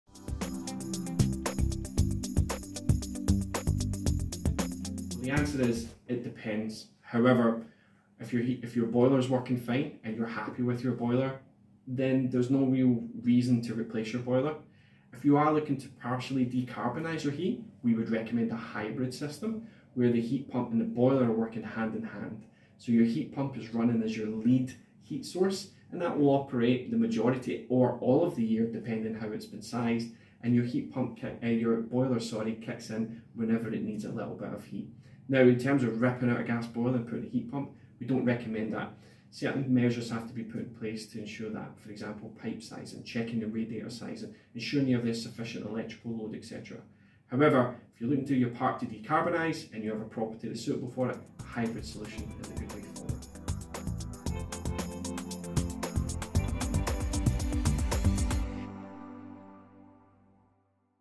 Can a heat pump replace your gas boiler?
Baxi's heat pump business development manager, Ryan Kirkwood, explains when you could install a heat pump or have a hybrid system using your existing gas boiler.